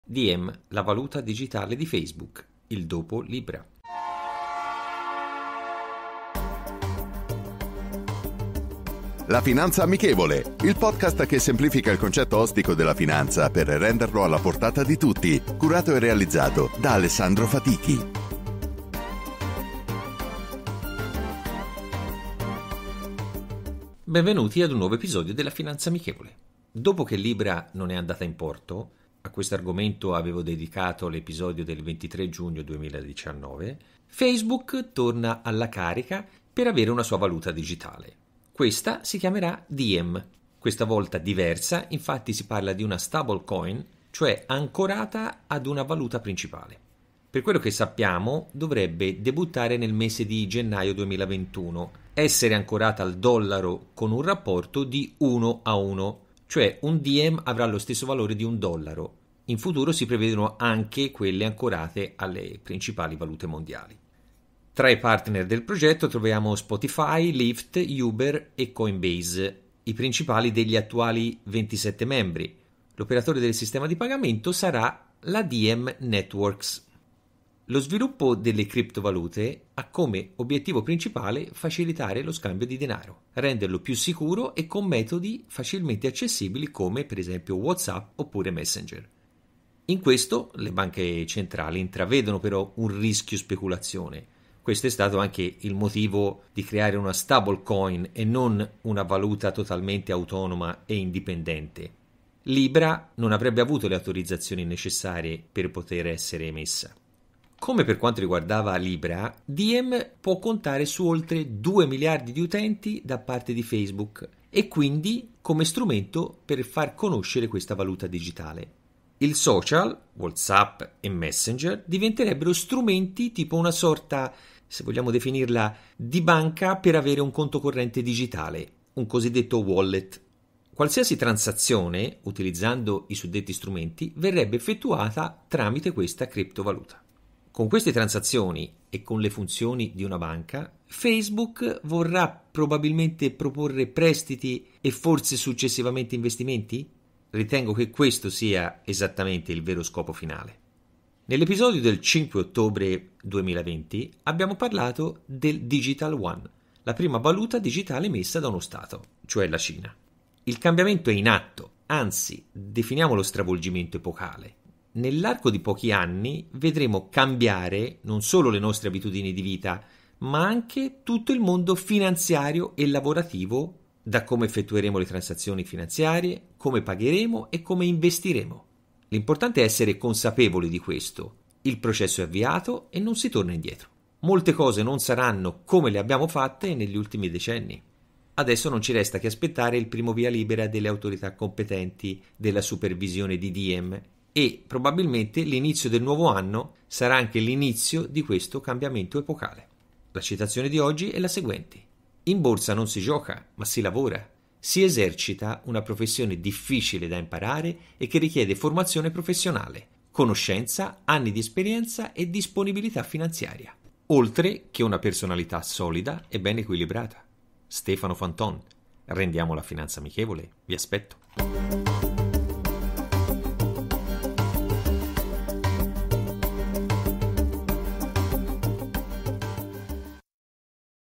Diem - la valuta digitale di Facebook, il dopo Libra
Dopo che Libra non è andata in porto, a questo argomento avevo dedicato l’episodio del 23 giugno 2019, Facebook torna alla carica per avere una sua valuta digitale. Questa si chiamerà Diem.
Questa volta diversa, in fatti si parla di una stablecoin cioè ancorata a una valuta principale.

"In borsa non si gioca ma si lavora, si esercita una professione difficile da imparare e che richiede formazione professionale, conoscenza, anni d’esperienza e disponibilità finanziaria, oltre che una personalità solida e ben equilibrata."
Stefano Fanton

Sigla di Eric Buffat

Per chi vuole acquistare il libro: